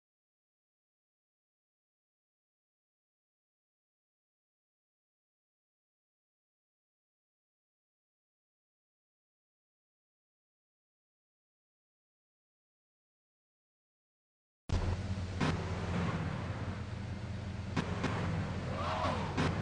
Why he got in it?
I was doing vigilante mission & this cop came up and got into my FBI rancher as if idk..After i put the godmod which has "Never Wanted" mod in it, cops became like a plain NPCs LoL..And looks like he missed his previous life as a cop LMAO..